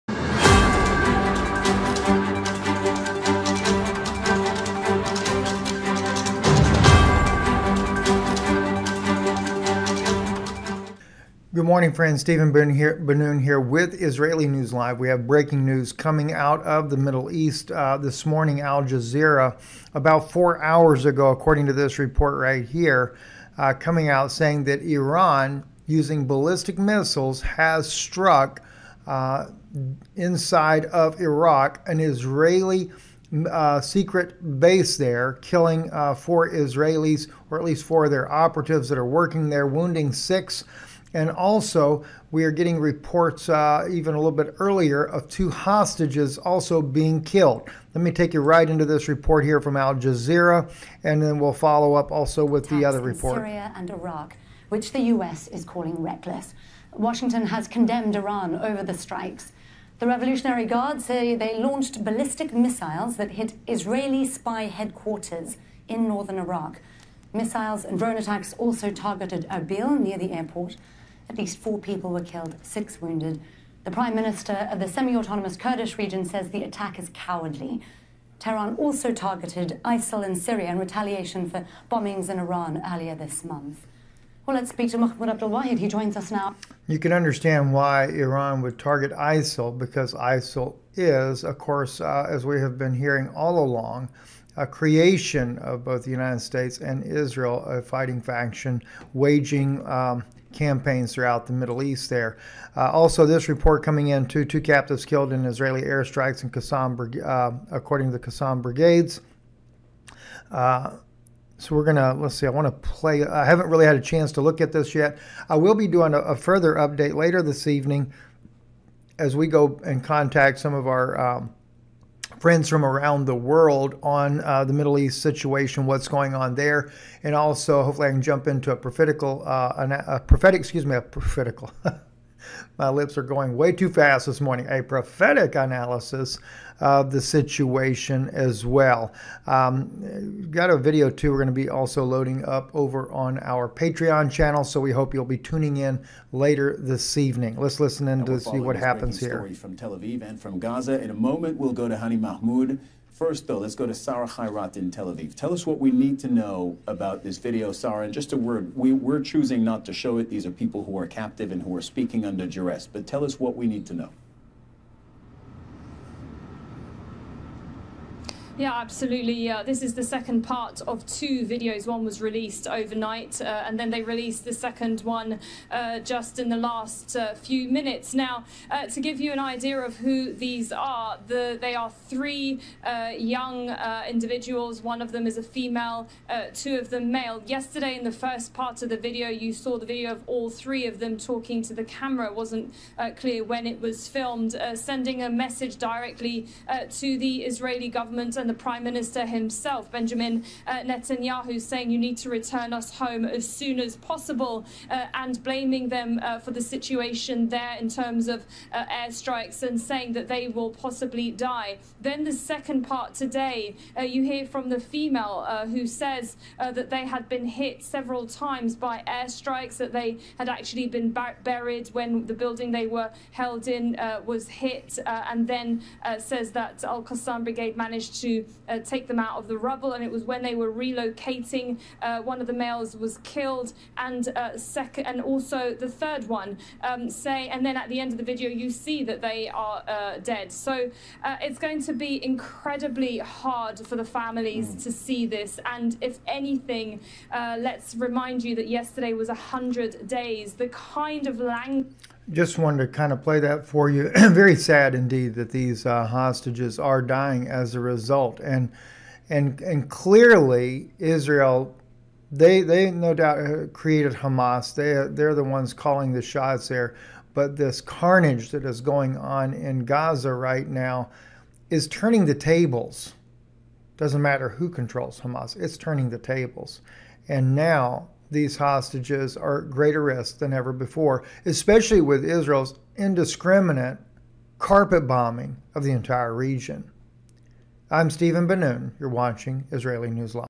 Breaking News: Iran Launches Cruise Missiles
Breaking News: Iran Launches Cruise Missiles striking Israeli Intelligence operations in Northern Iraq. Two Israeli Hostages killed by Israeli air strikes.